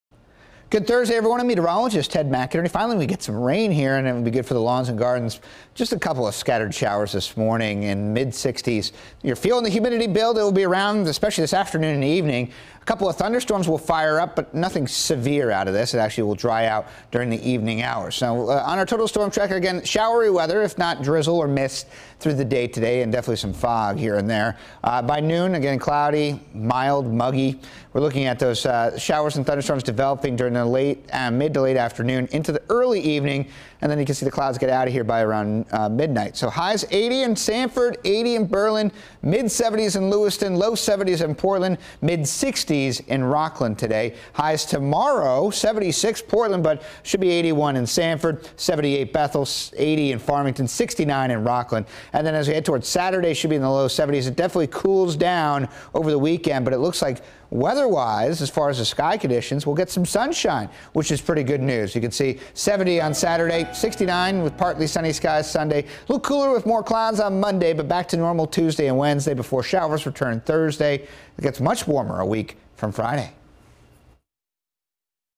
Afternoon storms and a drier weekend
How’s the weather looking for your Thursday? Get the latest Maine’s Total Weather video forecast from Meteorologist Ted McInerney.